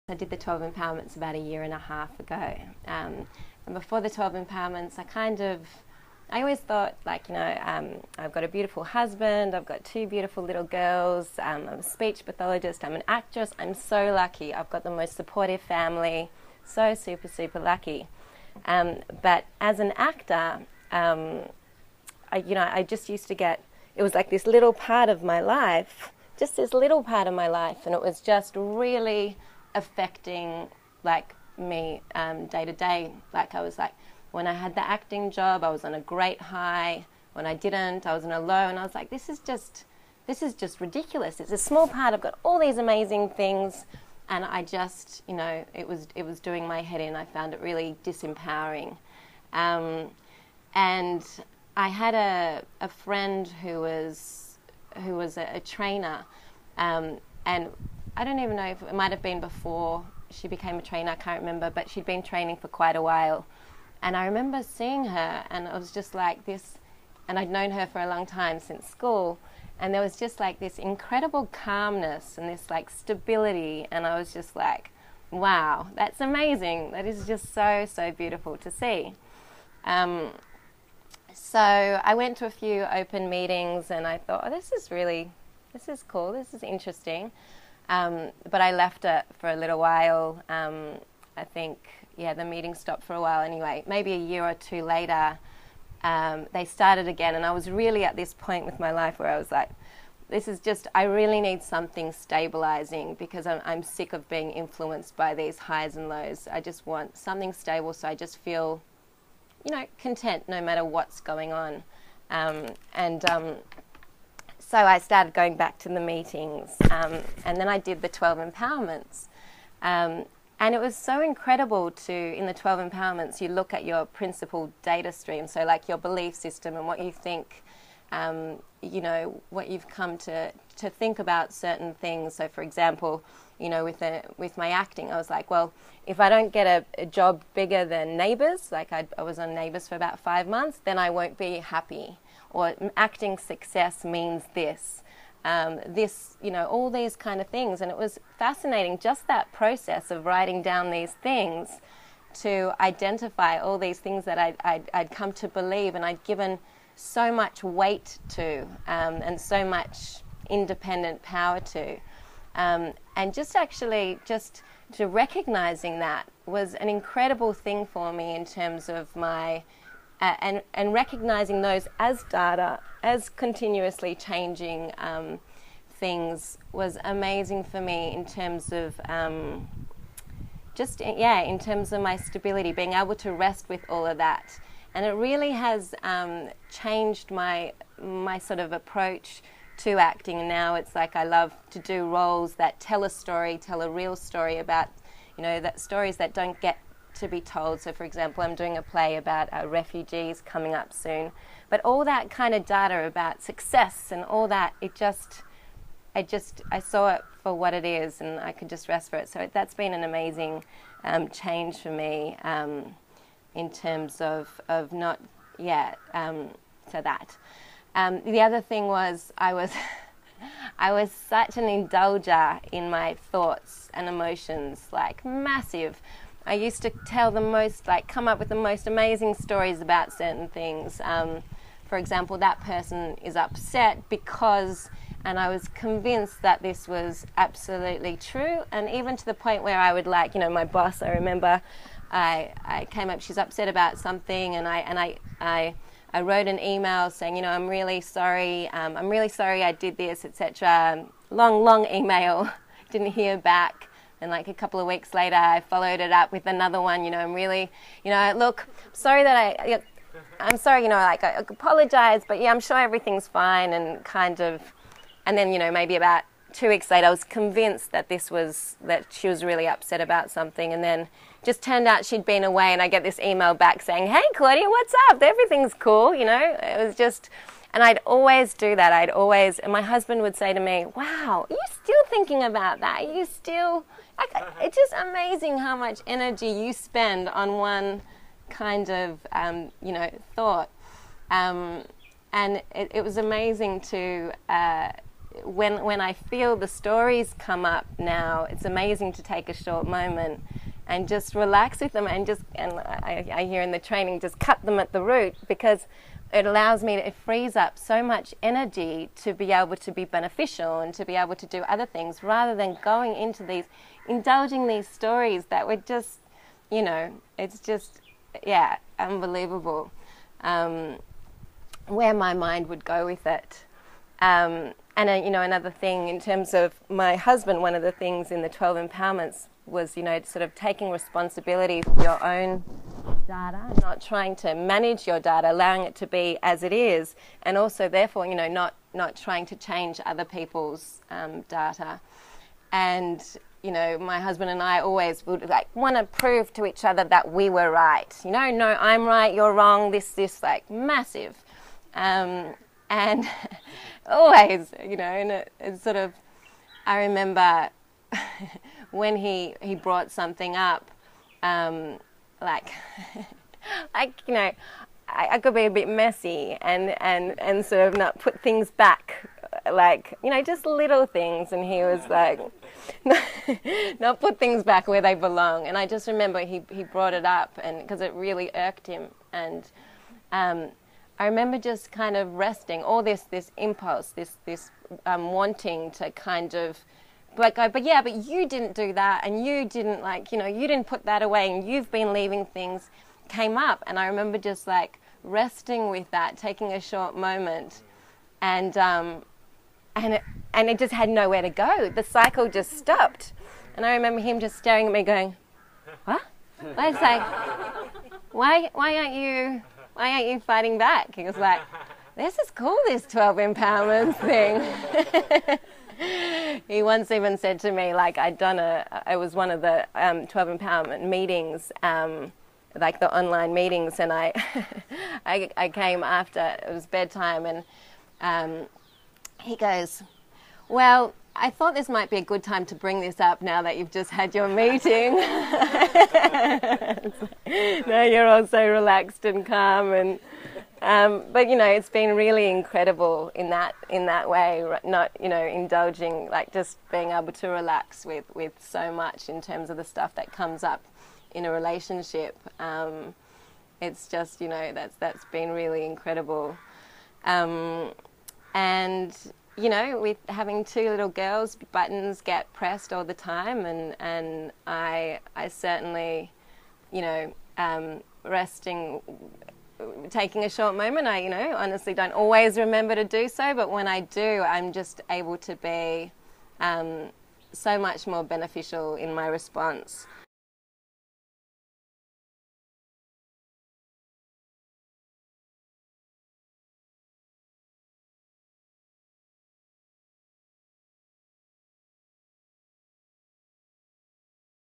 "I'm able to be so much more beneficial." Claudia, Australia | Balanced View Participant Share
Be introduced to how you can access consistent inner peace, wisdom and fulfilment in your life.